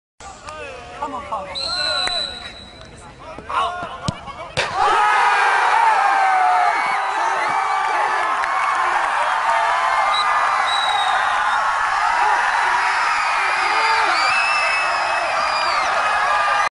Soccer Goalie Fail
This soccer goalie gets owned by his celebration